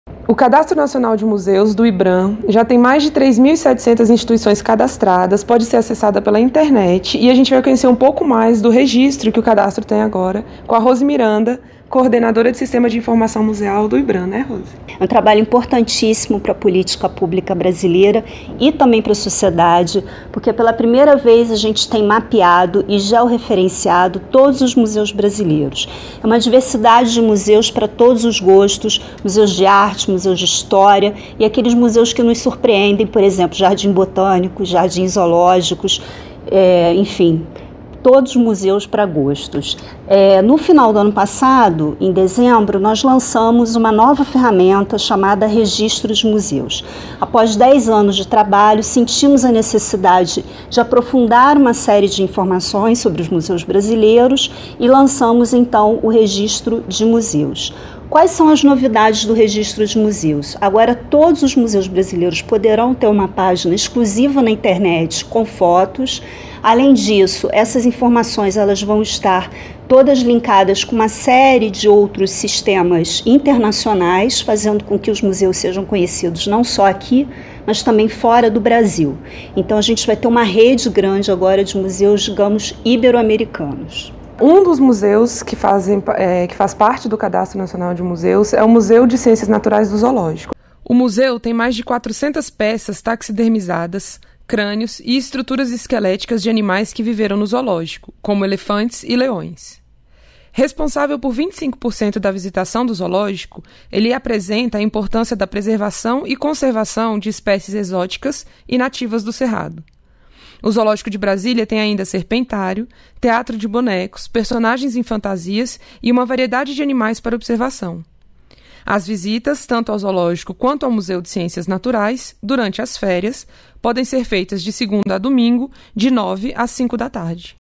Cadastro Nacional de Museus já tem 3.700 instituições cadastradas
ESPECIAL WEB - 10.01.17: O Cadastro Nacional do Instituto Brasileiro de Museus (Ibram) já tem 3.700 instituições cadastradas e pode ser acessado pela internet. A coordenadora de Sistemas de Informação Museal do Instituto, Rose Miranda, diz que pela primeira vez temos mapeado e georeferenciado todos os museus brasileiros.